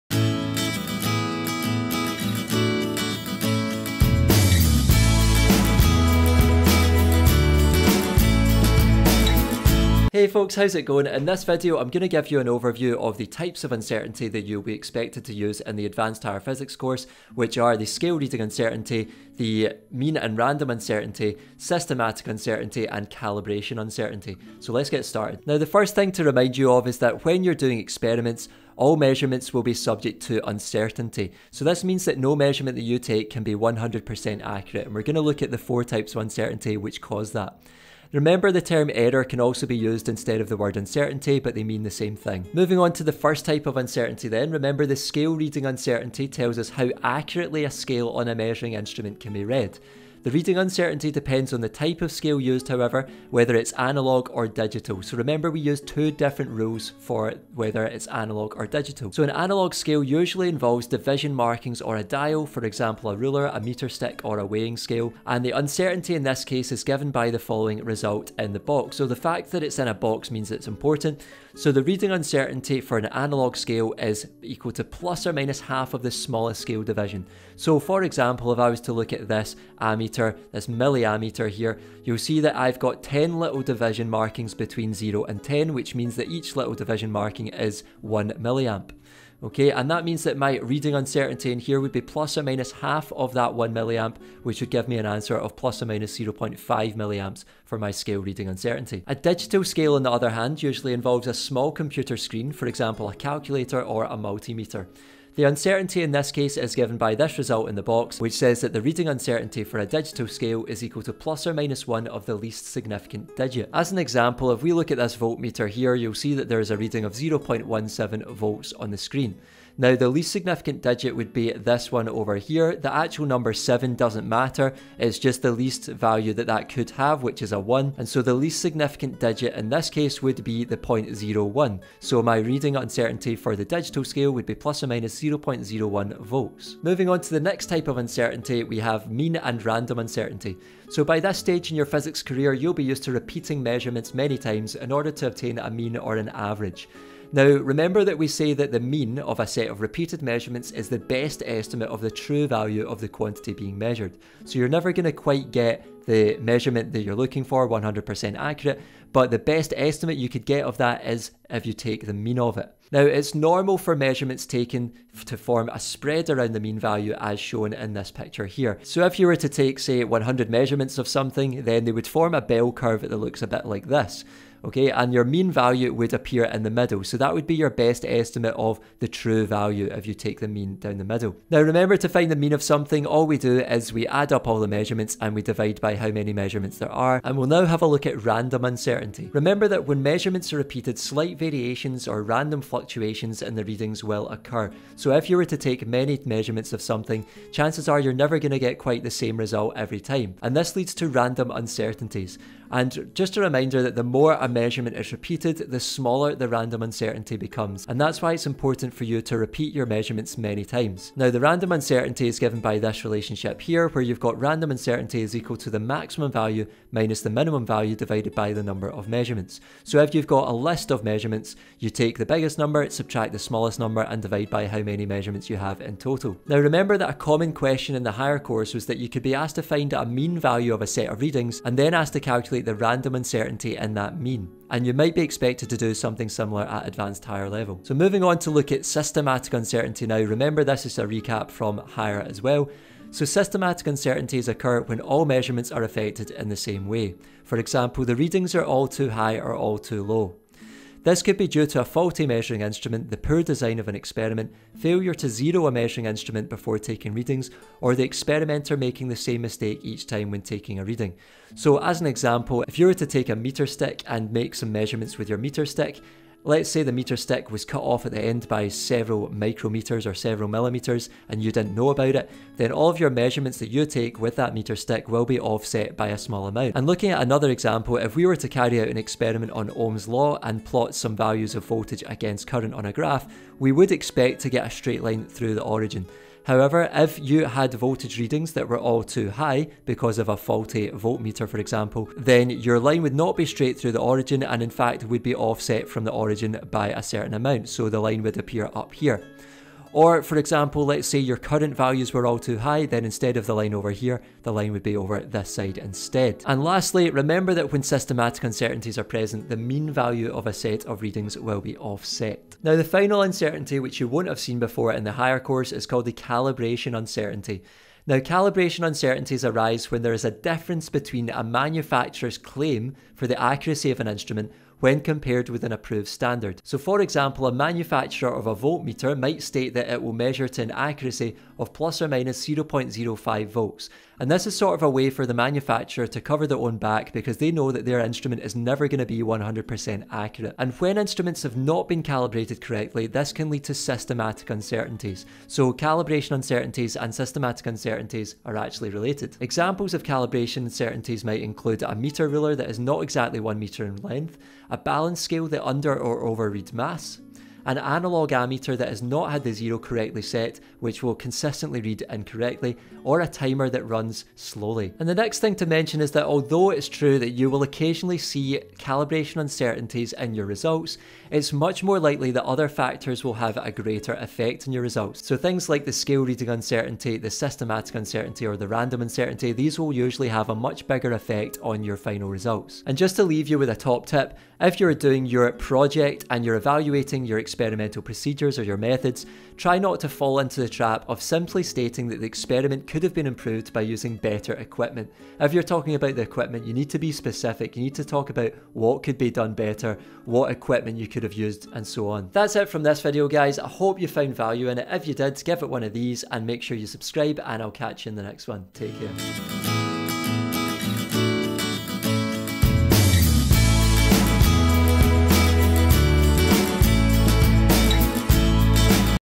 Advanced Higher Physics | Introduction | Types of Uncertainty | THEORY
An in-depth look at the types of uncertainty that you will come across in the Advanced Higher Physics course, specifically scale reading uncertainty, mean and random uncertainty, systematic uncertainty and calibration uncertainty.

⏱ Timestamps:
00:25 Introduction
00:42 Scale Reading Uncertainty
02:16 Mean and Random Uncertainty
03:57 Systematic Uncertainty
05:12 Calibration Uncertainty